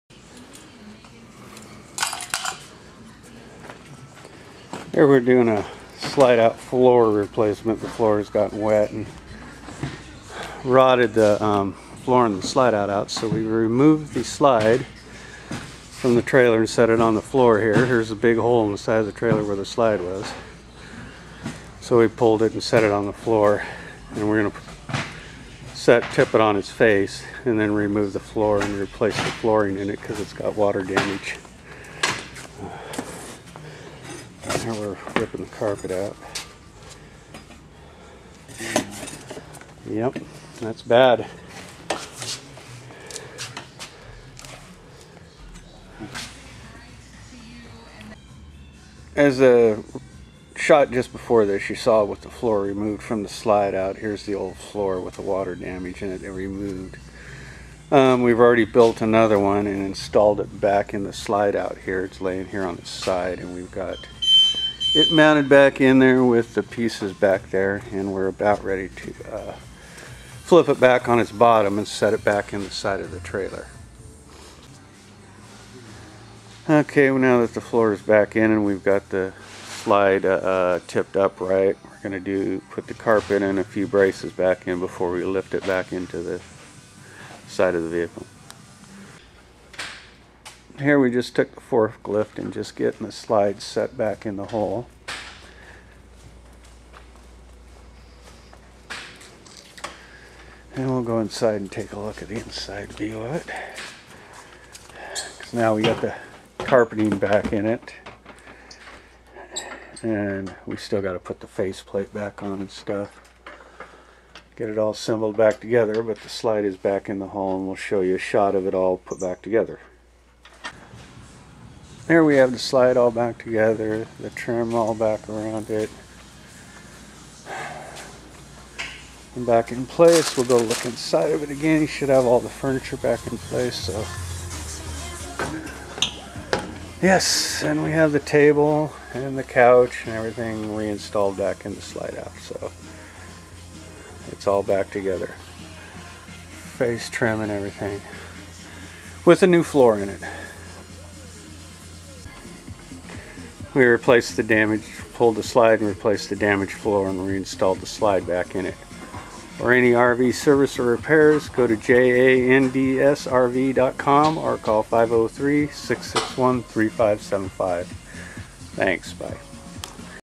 floor replacement on rv slide out
replacement of floor in slide out of travel trailer. remove slide out from trailer, replace floor and reinstall slide out.  jandsrv.com